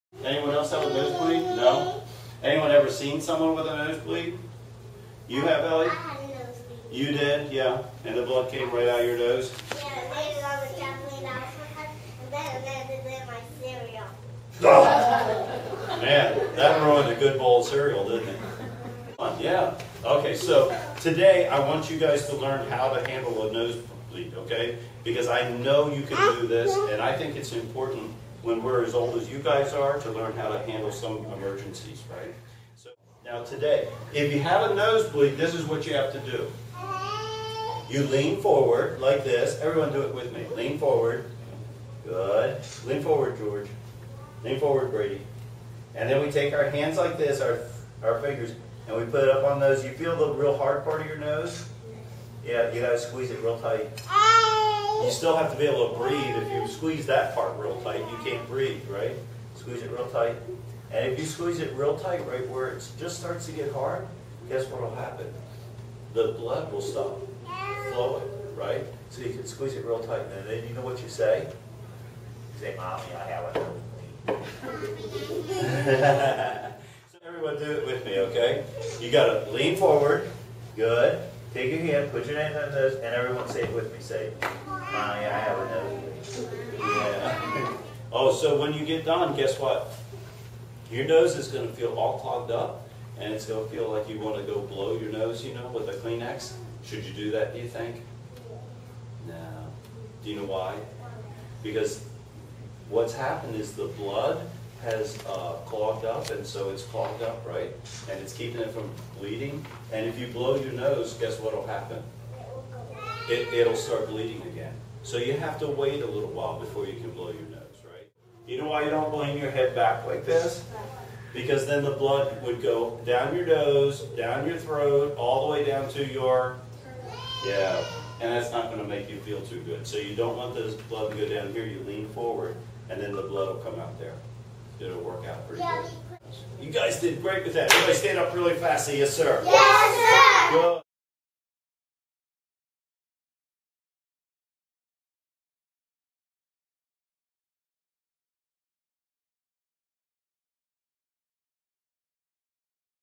4 Year Olds Learn About Nose Bleeds.m4v
How to handle an emergency can be taught even to 4 year olds.  This class learns how to handle their own nose bleed.